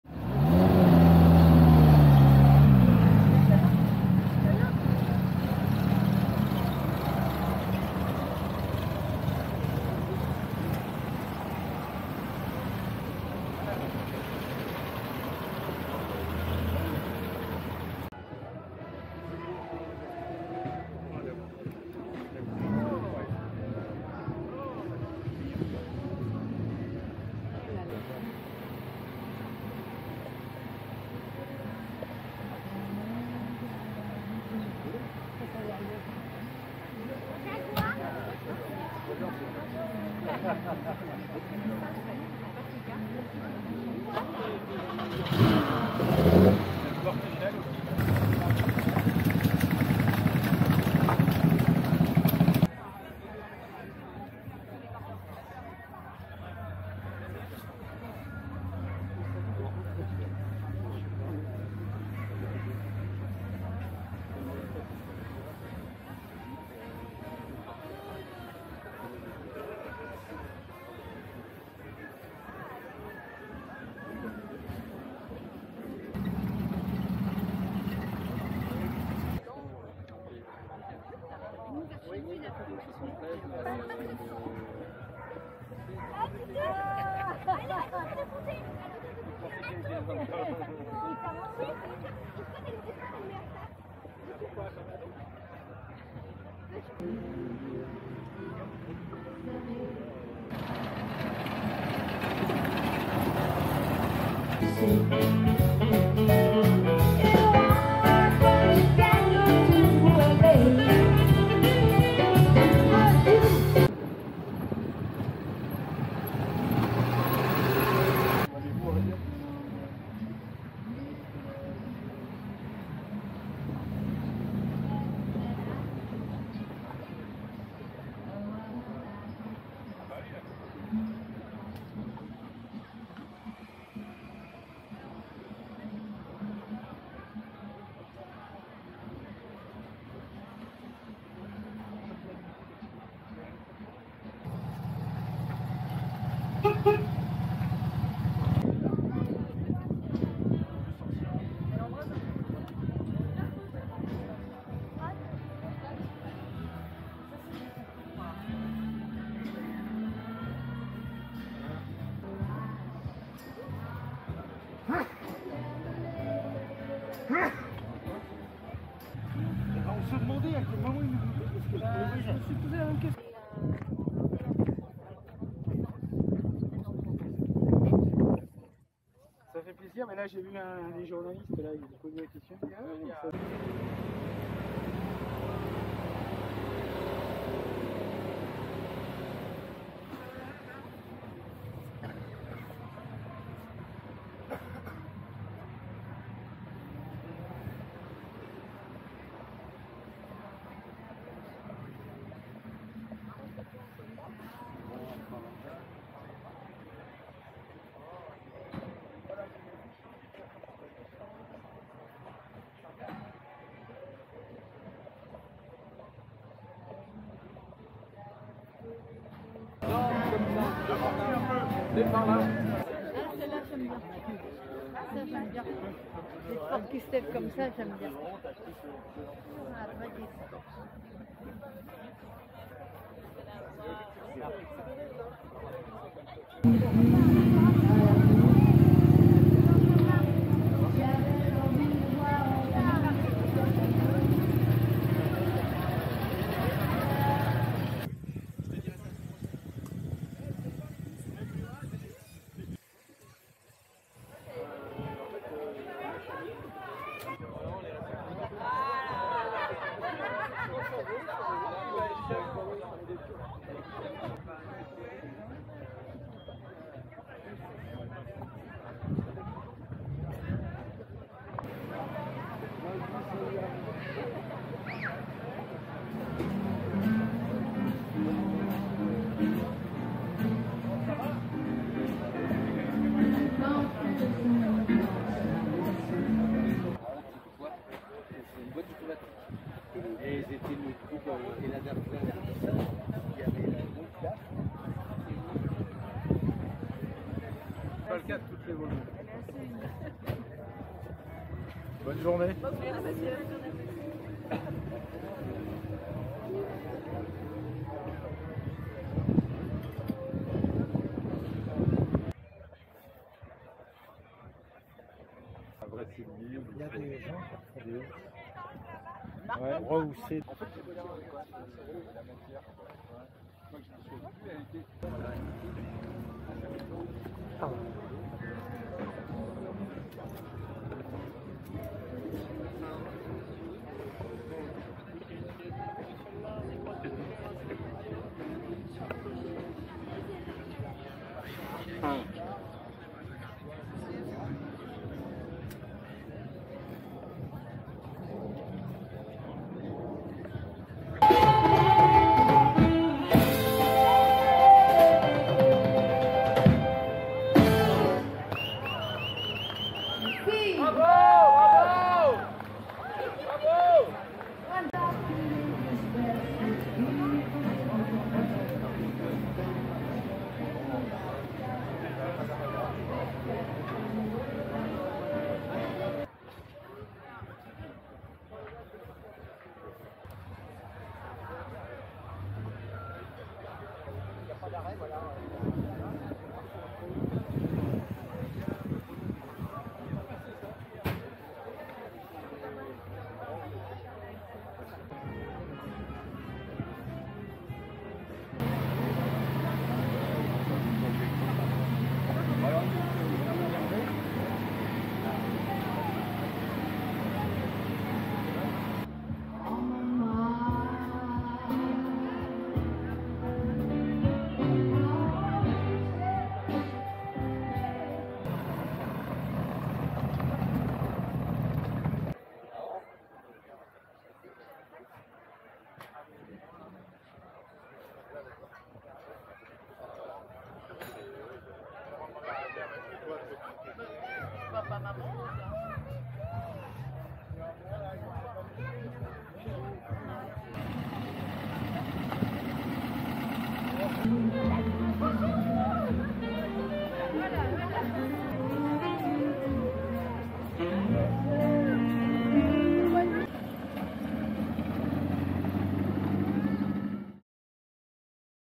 17e Traversée de Paris estivale, Meudon, Vincennes en Anciennes, voitures collection - Juin 2024
02:53 Puma GT (Brésil, chassis et moteur 1.6 l Volkswagen de 70 cv 1960~70)
04:47 DAF 55 coupé, moteur Cléon-Fonte Renault